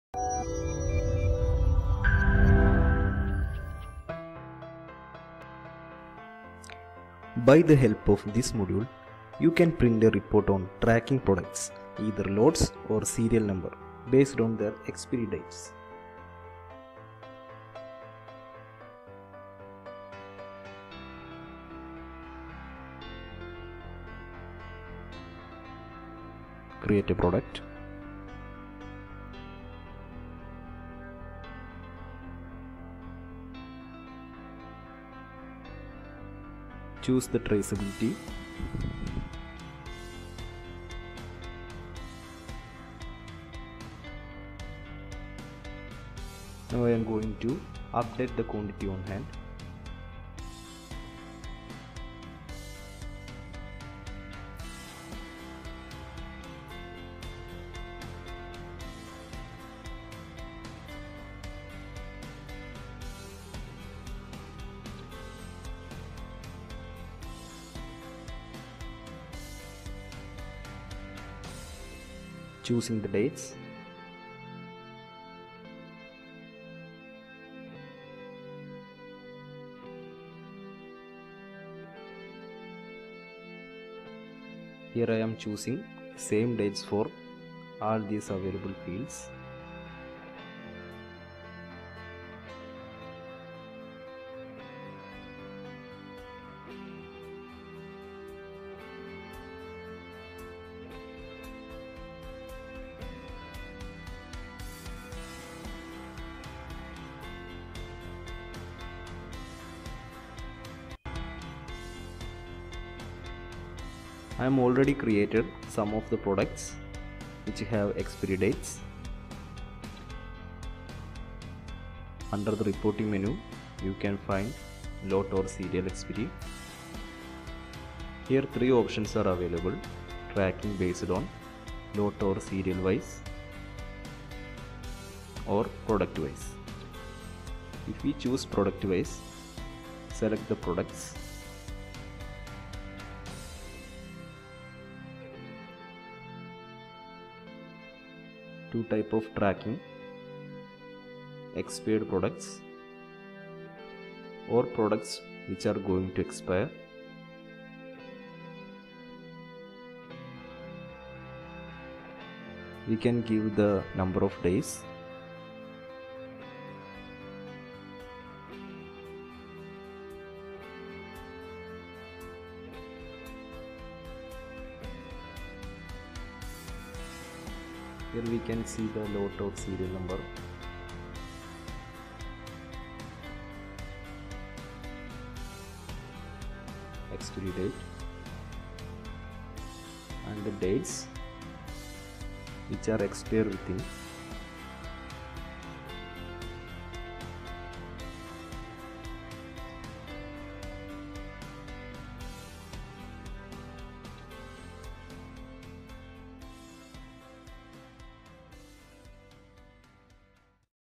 Lot and Serial Number Expiry Report in Odoo 12
This module helps you to print a report on tracking products (lot/serial) based on their expiry date.
One can filter their reports based on different criteria.